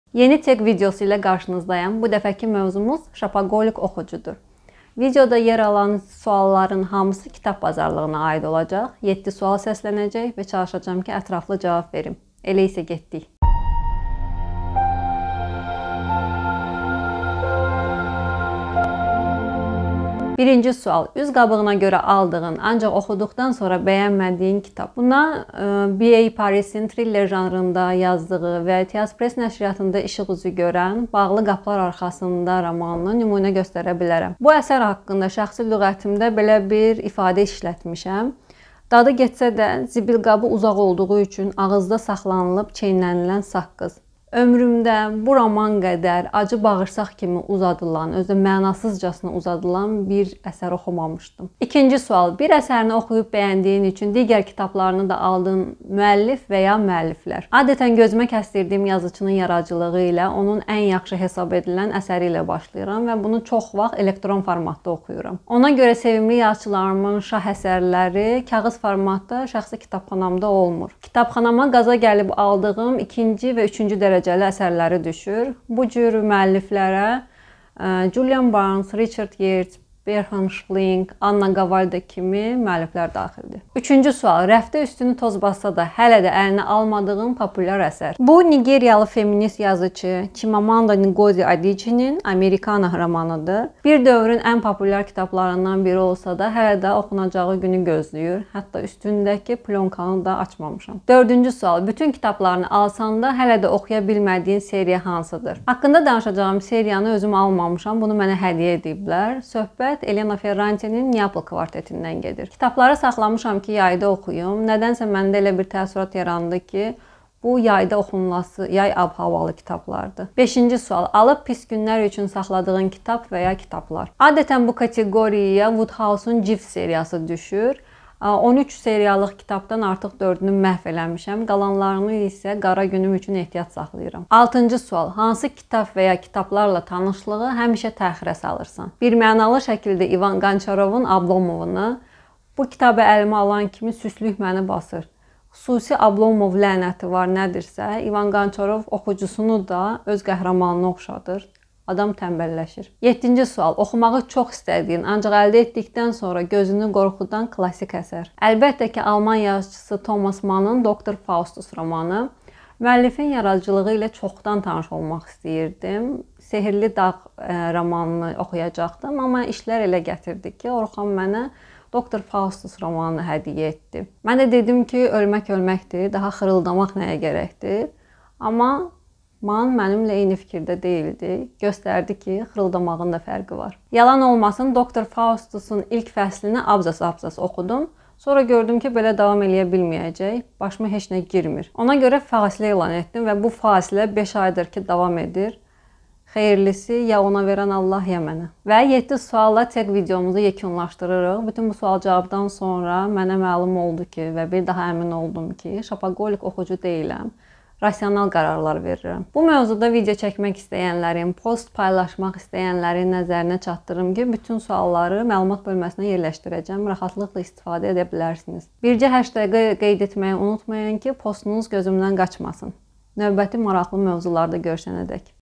Şopaholik oxucu - Teq videosu (The Shopaholic Tag)
Teq videosunda yer alan yeddi sualın köməyi ilə oxucular və ya alıcılar sırasında olduğumu özüm üçün aydınlaşdırmağa çalışdım.

Suallar:

1. Üzqabığına görə aldığın, ancaq oxuduqdan sonra bəyənmədiyin kitab(lar)?
2. Bir əsərini oxuyub bəyəndiyin üçün digər kitablarını da aldığın müəllif(lər)?
3. Rəfdə üstünü toz bassa da, hələ də əlinə almadığın populyar kitab(lar)?
4. Bütün kitablarını alsan da, hələ də oxuya bilmədiyin seriya?
5. Alıb pis günlər üçün saxladığın kitab(lar)?
6. Hansı kitab(lar)la tanışlığı həmişə təxirə salırsan?
7. Oxumağı çox istədiyin, lakin əldə etdikdən sonra gözünü qorxudan klassik əsər?